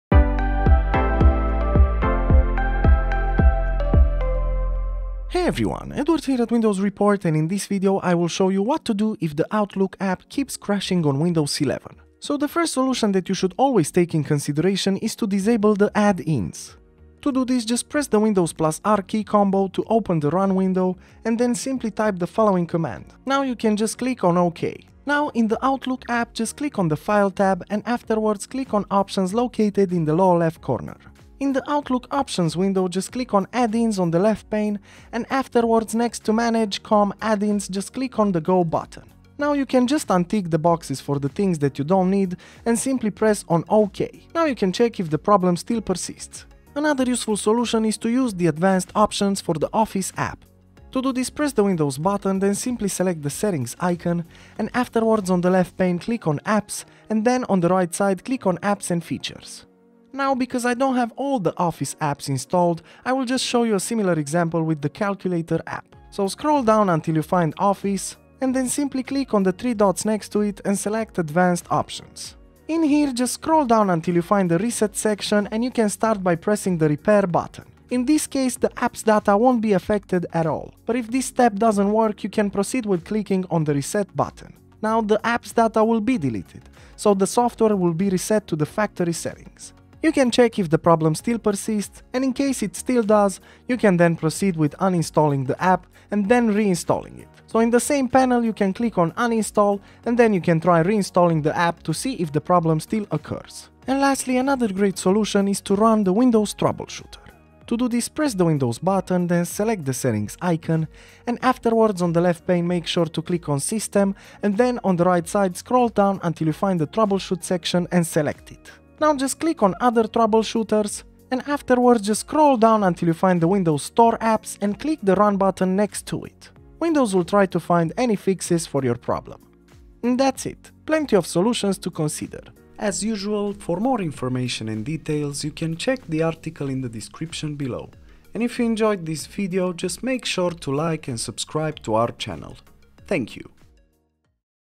Outlook keeps crashing on Windows 11? Check these fixes!
| ⚙️Try Fortect to fix and optimize your PC like a Pro

The popular mail app can sometimes fail. And it's normal - but you should be frustrated. So let's try a few solutions to solve this issue!

0:00 - Intro
0:14 - Disable add-ins
0:47 - Use the Advanced Options
1:45 - Run the Windows troubleshooter